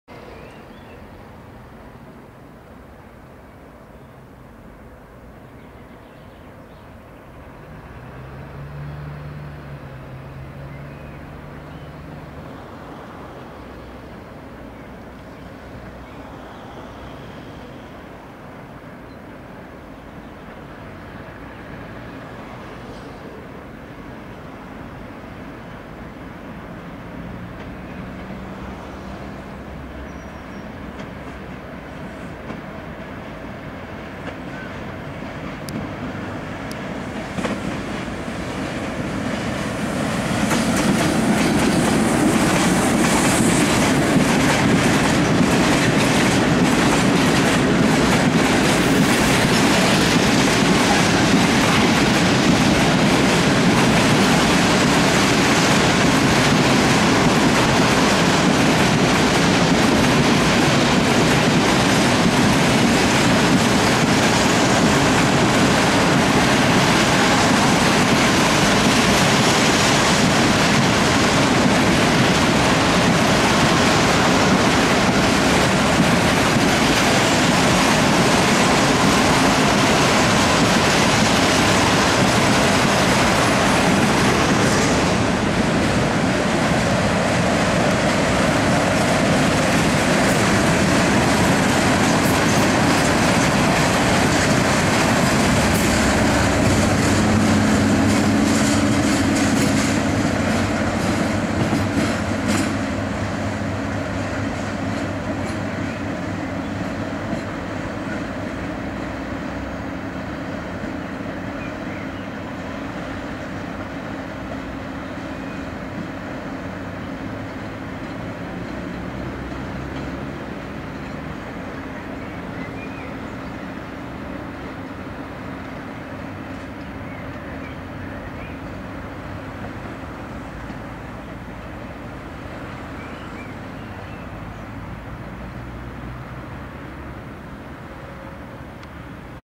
Czeska E182 ČD-129 +cysterny +SM 42 w wąwozie koło ZOO ,Płock
Płock , zapewne wąwóz sztuczny wykopany jeszcze przed wojną. Za zakretem wjedzie na most kolejowo drogowy, widac go tez z Gagarinem na innym moim fimie.